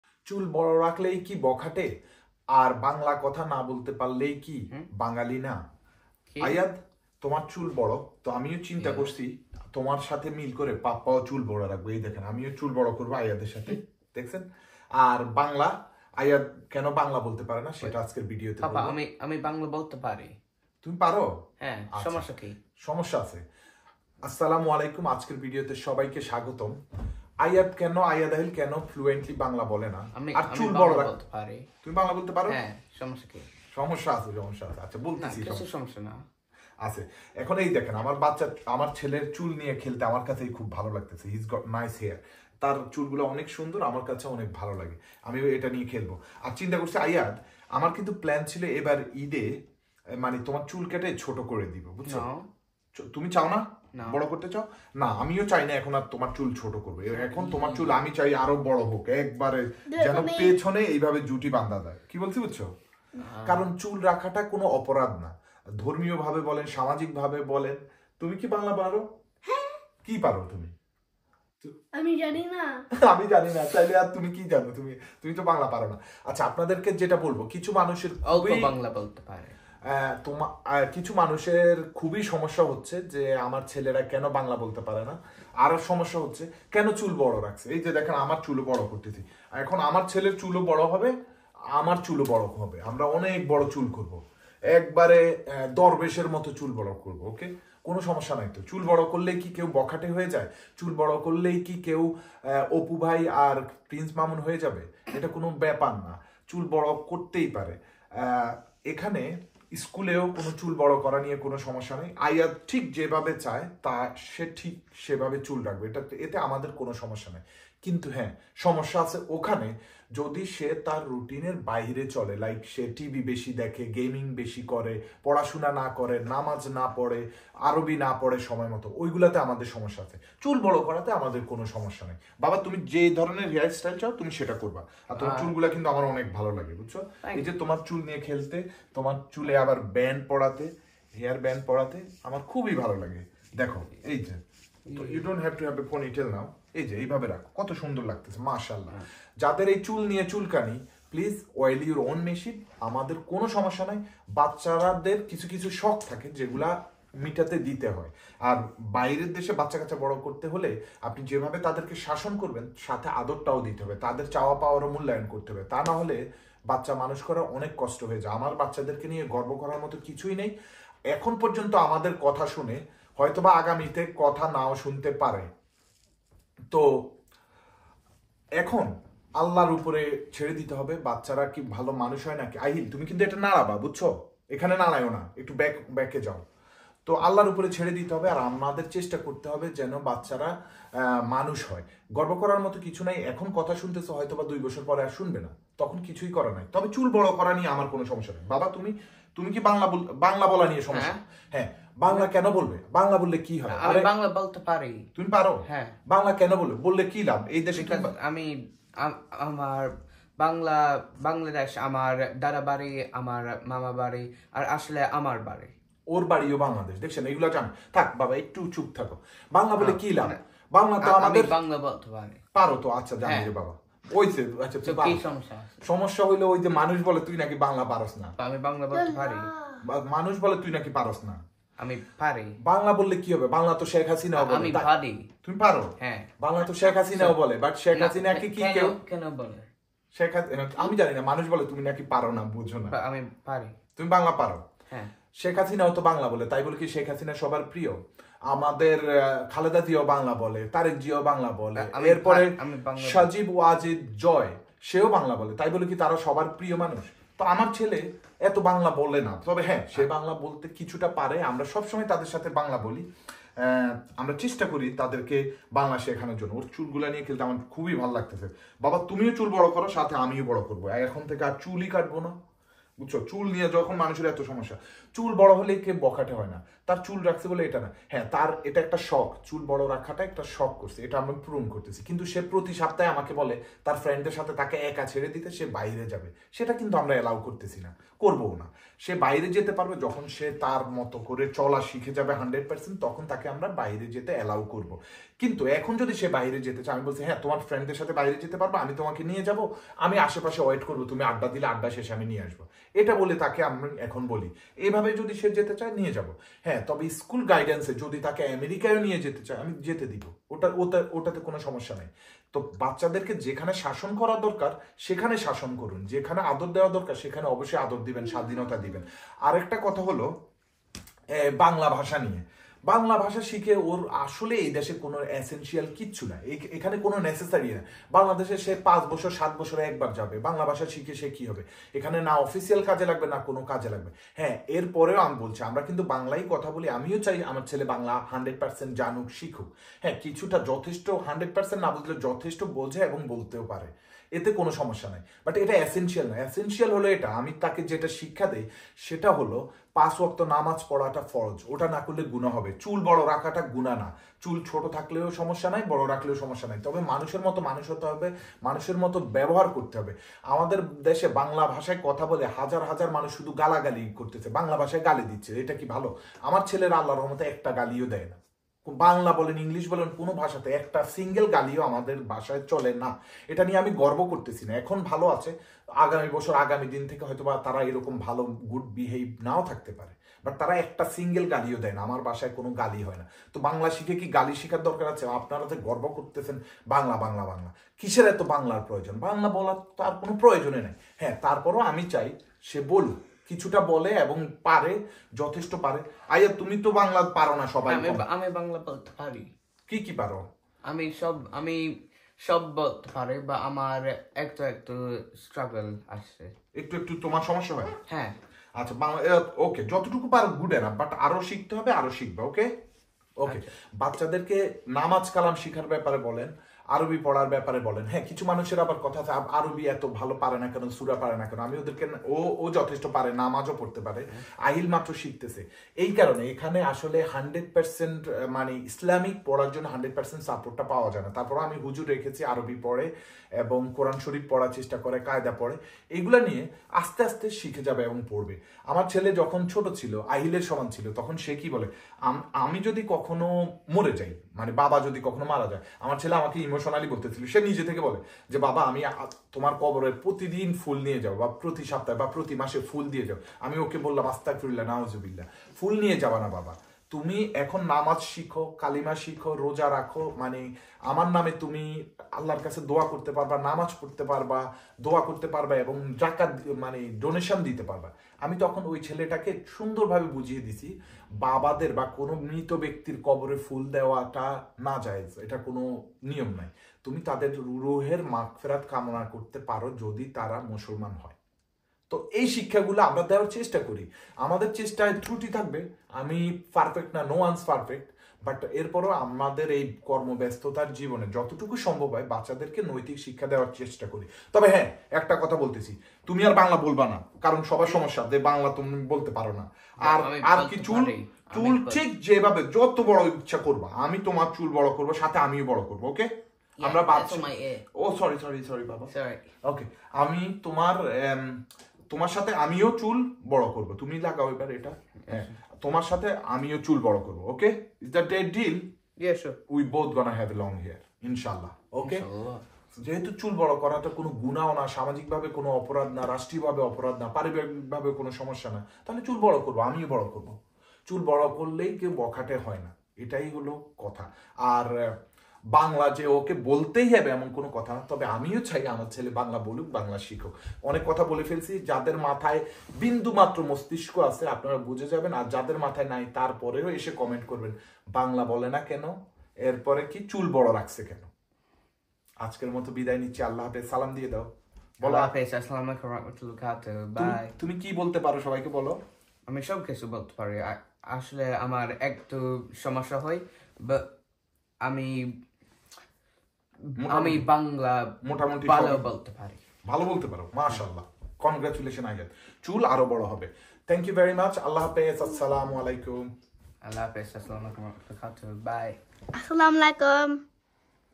চুল বড় রাখলেই কি বখাটে? বাংলা বলতে না পারলেই কি বাঙালি না?|JF DISHA VLOGS||Bengali Vlogs
চুল বড় রাখলেই কি বখাটে? বাংলা বলতে না পারলেই কি বাঙালি না?